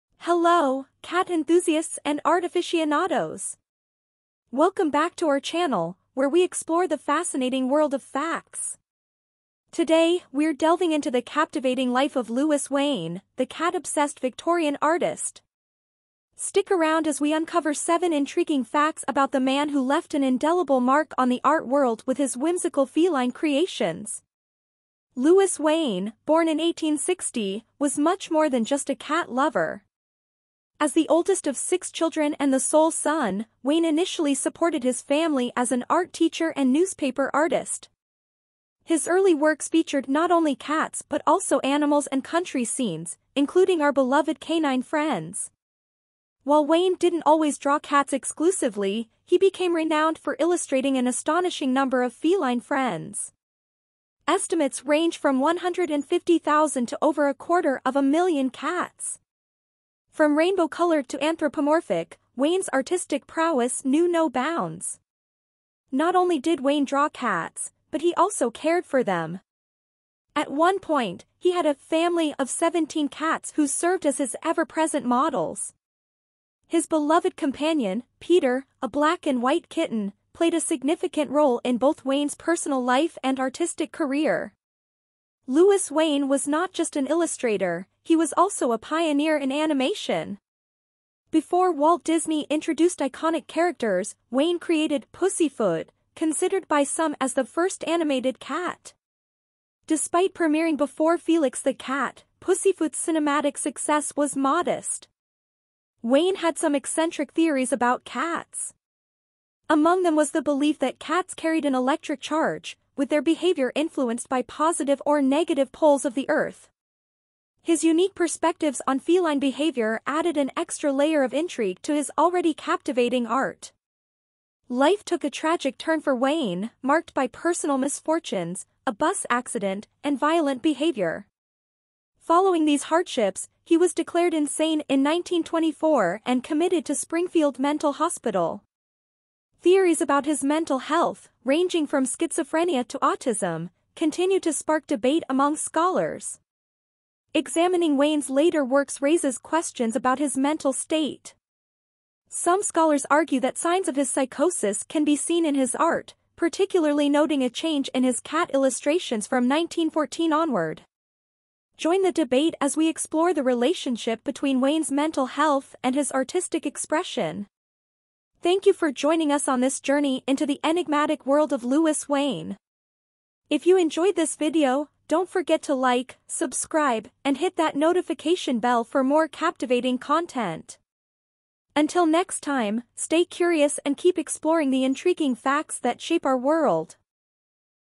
Cat-Tales Unleashed: 7 Facts You Don t Know About Louis Wain's Journey
Dive into the whimsical world of Louis Wain, the Victorian artist whose passion for cats transcended the ordinary. Join us on 'Whiskers and Wonders' as we unveil 7 captivating facts about Wain's extraordinary life, from his diverse artistic talents and prolific cat illustrations to his pioneering role in animation. Explore the unique theories he held about feline friends, witness the tragic turn in his life, and examine the intriguing connection between his art and mental health. Don't miss this enchanting journey into the enigmatic mind of Louis Wain, where each stroke of the brush tells a tale of creativity, companionship, and the enduring legacy of a true art maestro.